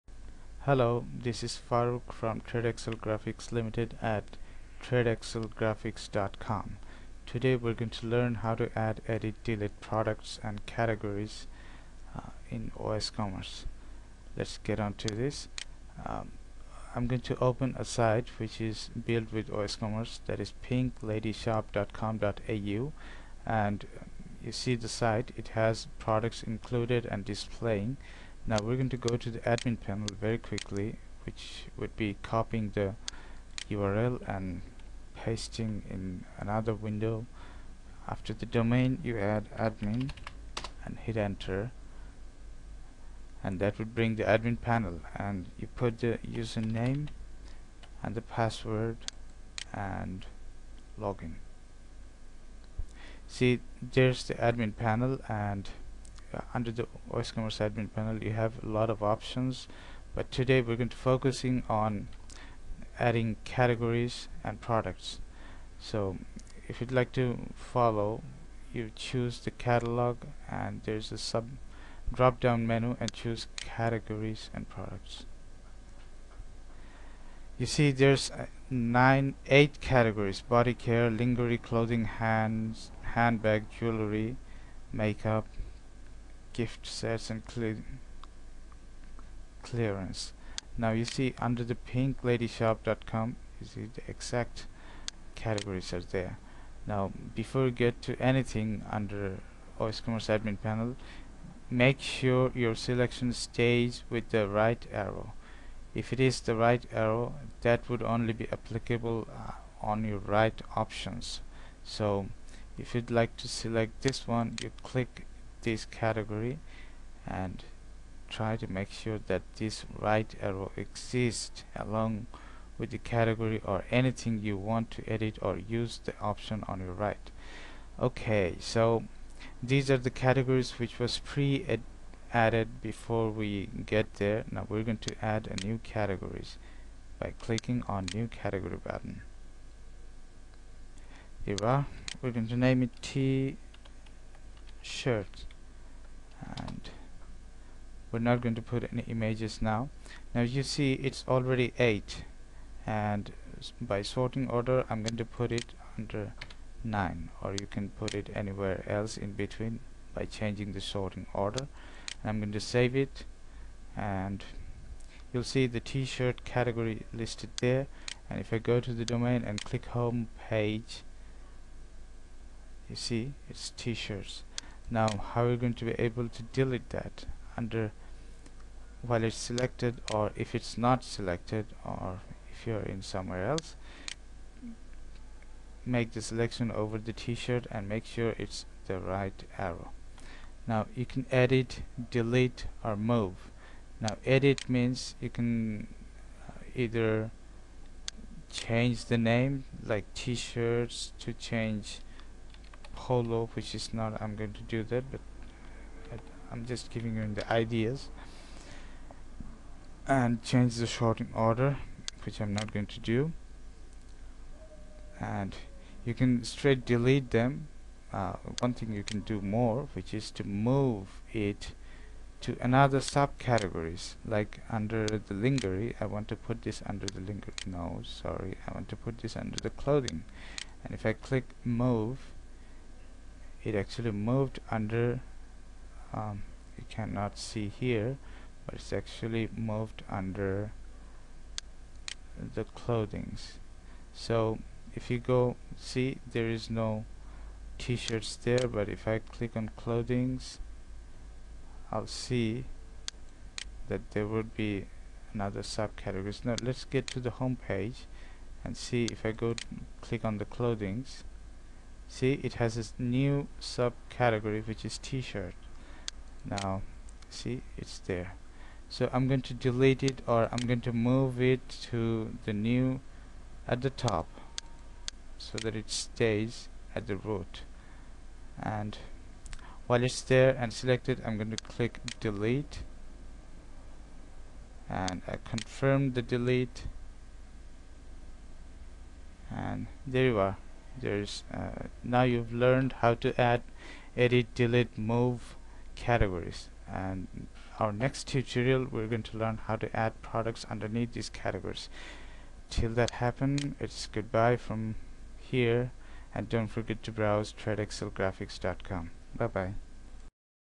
Oscommerce Tutorial Part 1 - tradexcelgraphics.com
We Tradexcelgraphics.com proudly presenting our first video tutorial about oscommerce, and there are still lot more to come. In this tutorial we have explained how to add edit and delete catagory in oscommerce. In our next tutorial we will be explain how to add edit delete Products. we hope you could join us.